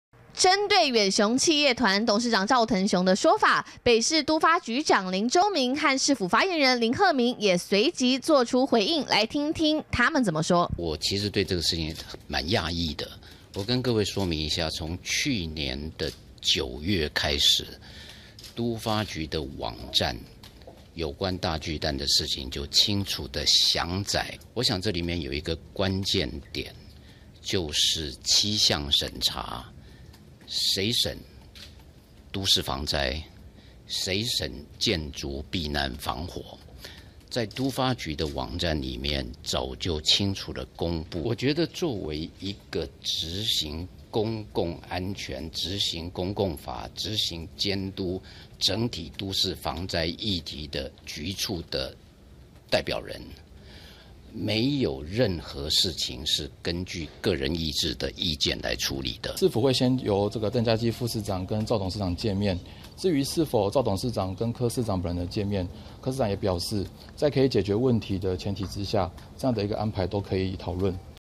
【2016.07.27】趙藤雄批蒙蔽柯P 林洲民:嚴重指控
針對遠雄企業團董事長趙藤雄的說法，北市都發局長林洲民，和市府發言人林鶴明，也隨即做出回應，來聽聽他們怎麼說。完整內容 , FB , Twitter , G+